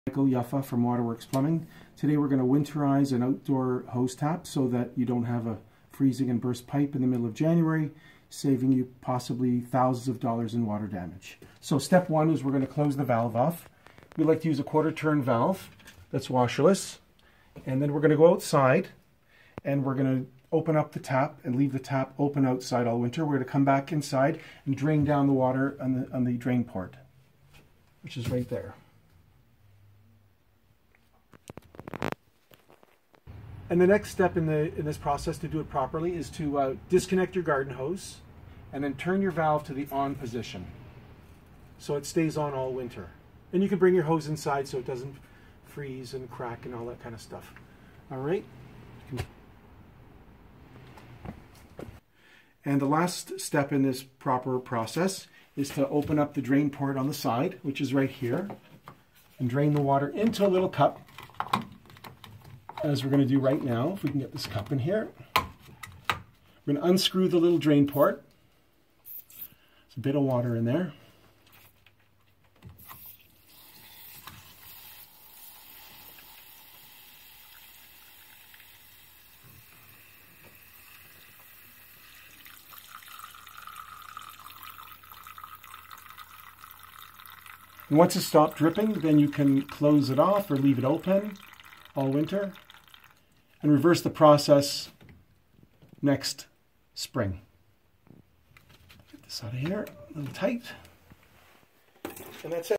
How to winterize outdoor garden tap for the winter in Toronto
This will save you flood damage from the first and frozen water line in the middle of winter, especially with our Winters in Toronto